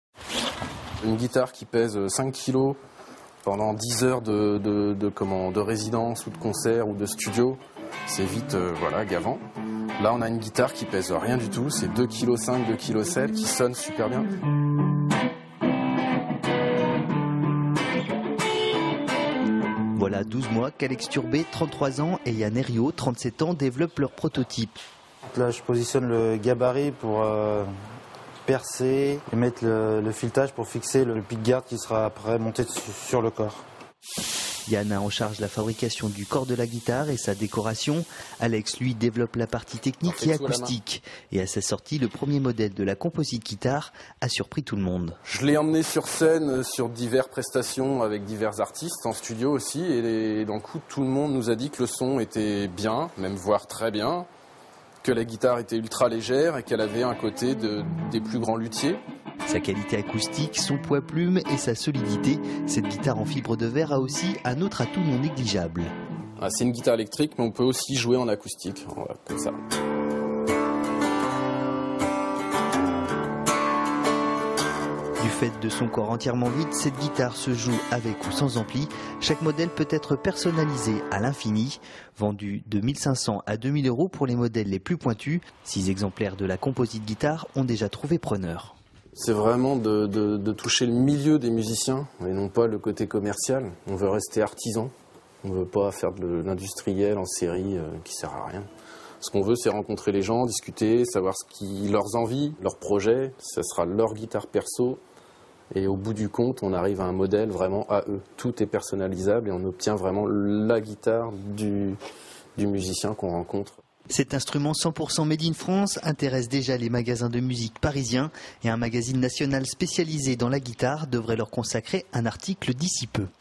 Musique : Découverte de la Kompozit guitare (Vendée)
Sites webs/blogs, copiez cette vidéo avec le bouton "Intégrer" !
 Rencontre avec Alex Turbé et Yann Airiau qui ont créé la Kompozit, une guitare fabriquée en fibre de verre.